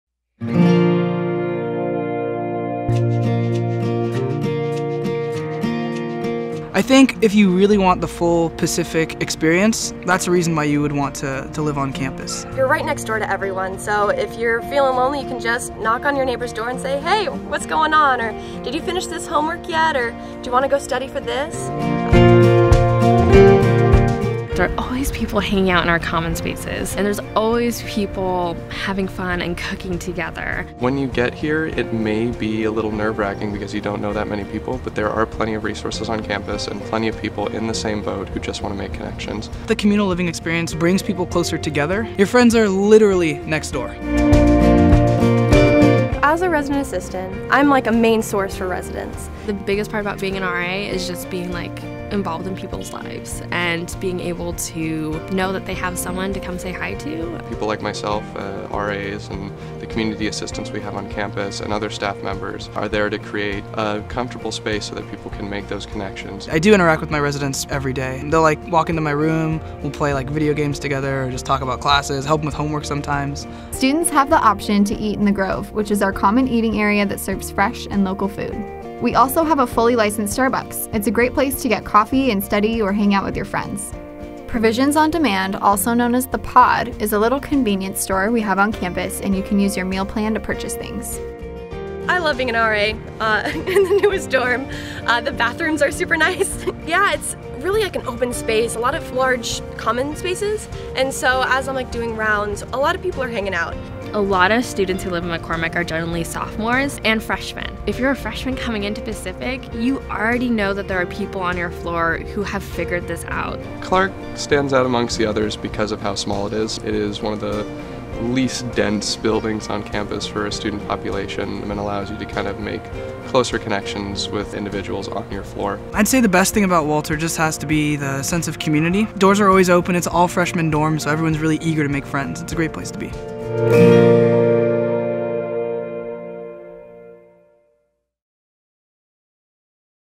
Freshmen Housing at Pacific
Meet Resident Assistants and see where you will be living your first year at Pacific!